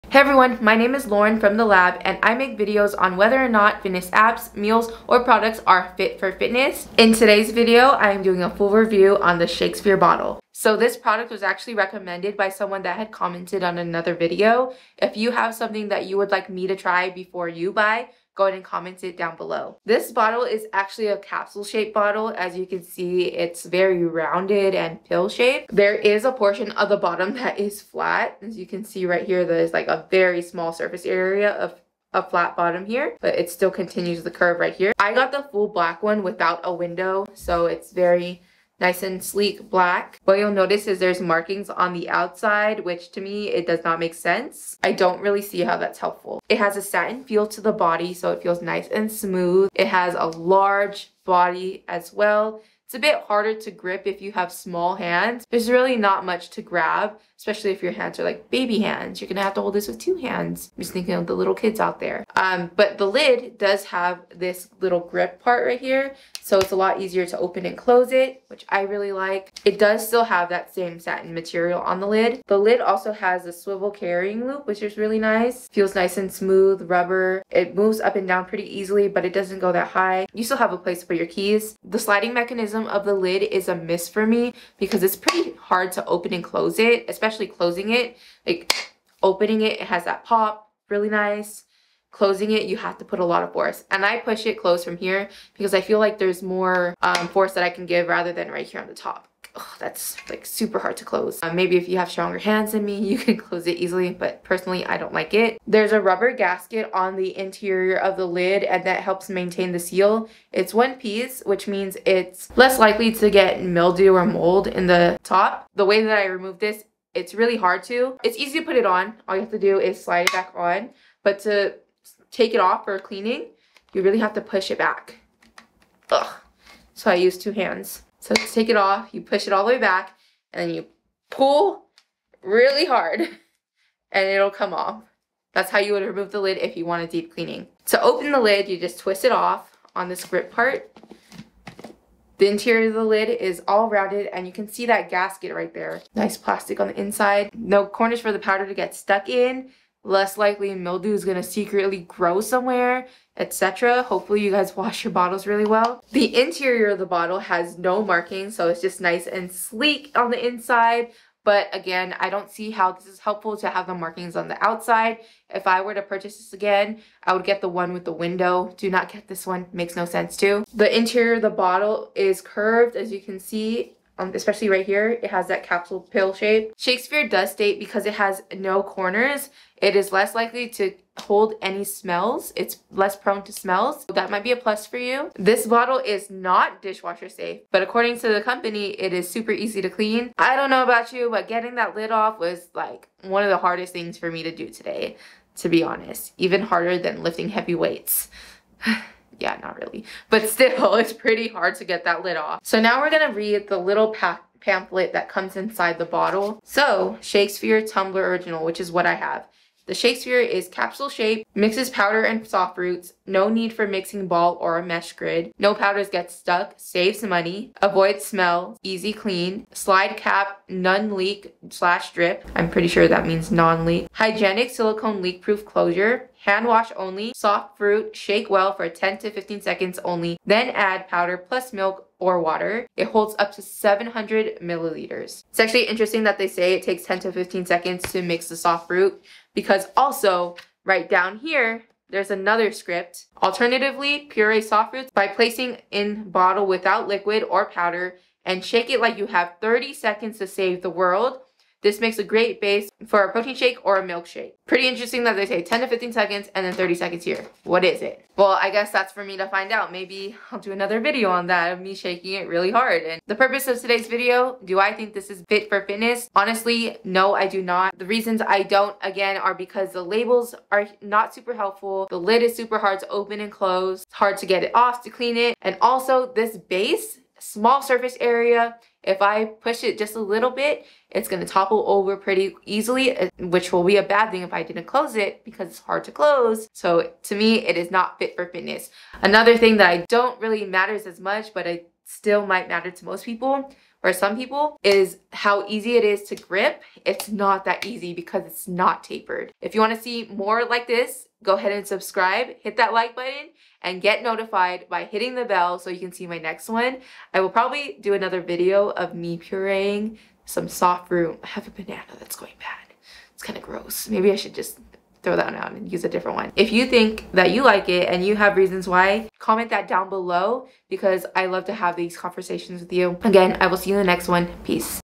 ShakeSphere Shaker Bottle Review - WORTH BUYING FOR WORKING OUT?
Lauryn deep dives on the ShakeSphere Shaker cup which is a pill shaped bottle. Watch and listen to find out if this product is Fit for Fitness.